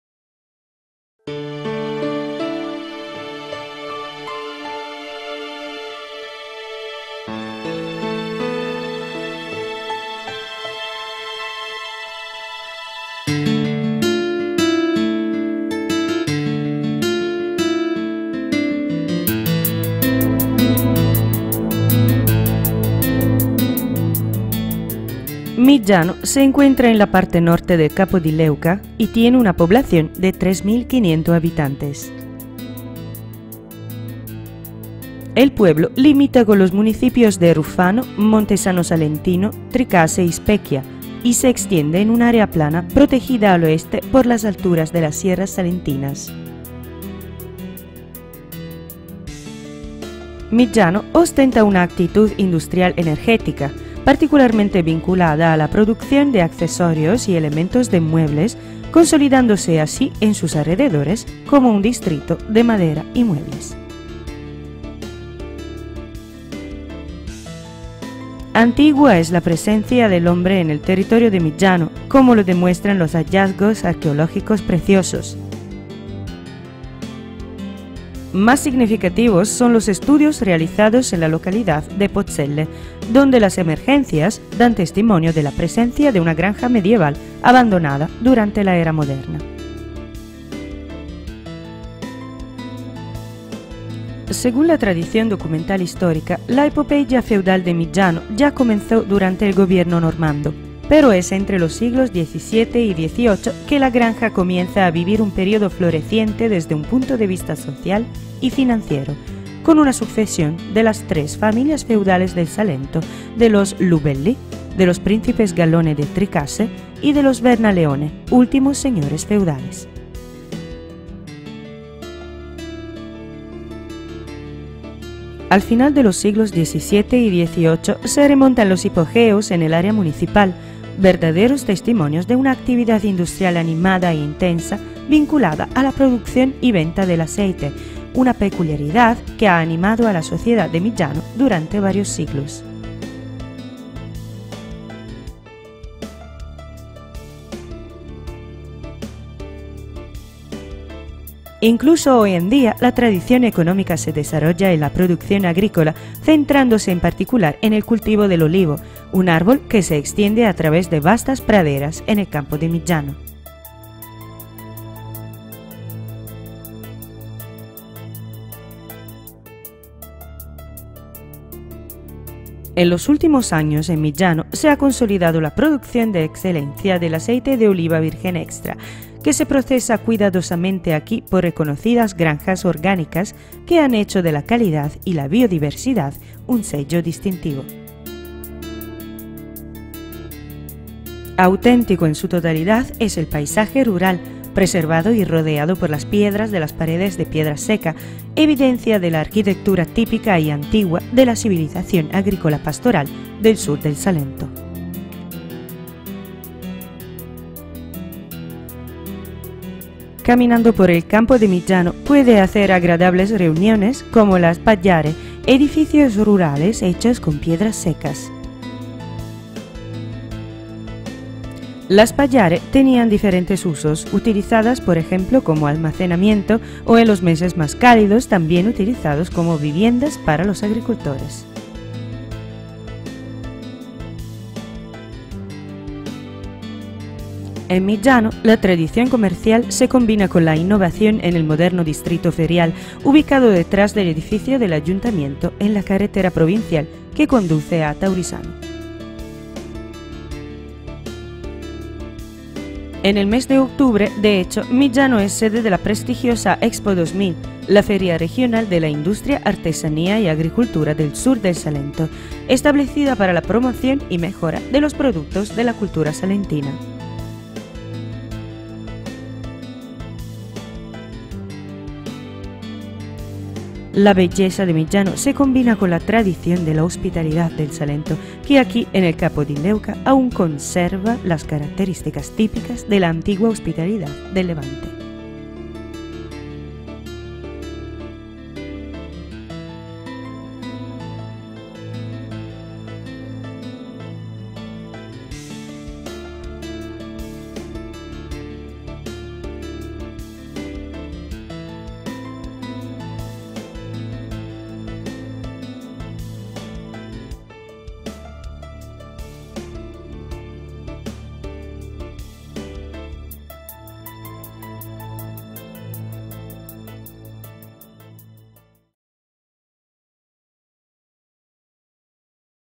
Ciudad de Miggiano - Introducción
Miggiano ostenta una actitud industrial energética, particularmente vinculada a la producción de accesorios y elementos de muebles, consolidándose así en sus alrededores como un distrito de madera y muebles.
Antigua es la presencia del hombre en el territorio de Miggiano, como lo demuestran los hallazgos arqueológicos preciosos.